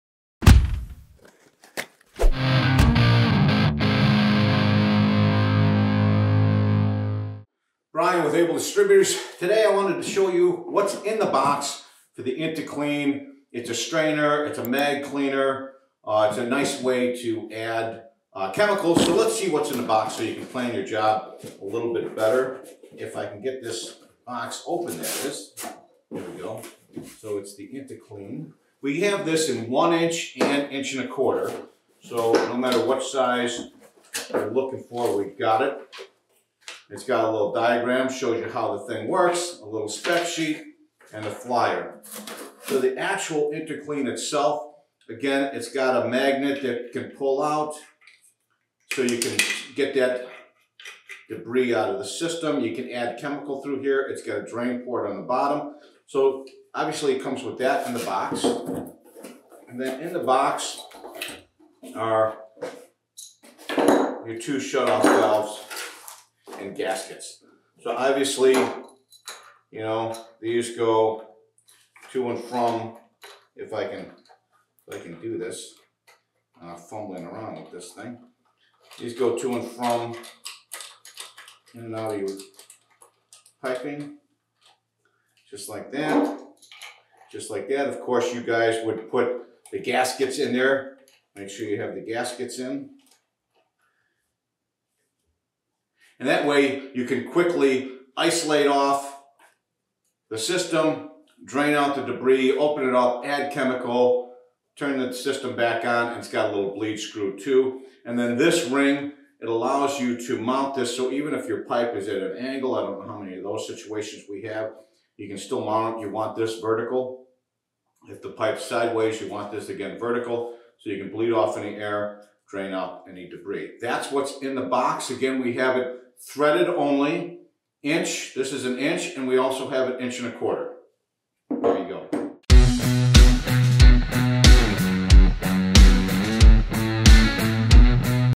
Zilmet IntaKlean 2 Magnetic Dirt Separator: What's in the Box?
The IntaKlean2 is a Multi functioning Magnetic Dirt Separator filter for Hydraulic Circuits.
Available in 3/4", 1" and 1 1/4" sizes
Can be mounted vertically, horizontally or diagonally
Dual removable stainless steel mesh filters
11,000 Gauss magnet in dry-well
Easy access for adding up to 16 oz of treatment fluids at a time